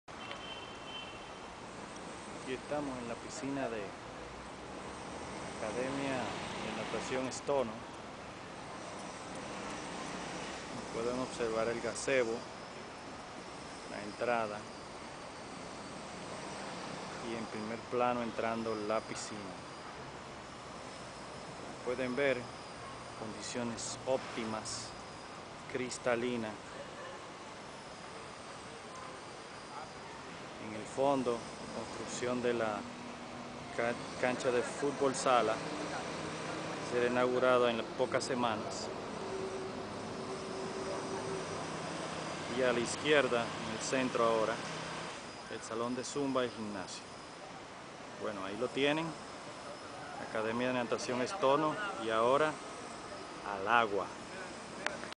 Ambiente Academia Stono Natacion
Esta es la piscina donde estamos ofreciendo las clinicas de natacion para nadadores de todos los niveles.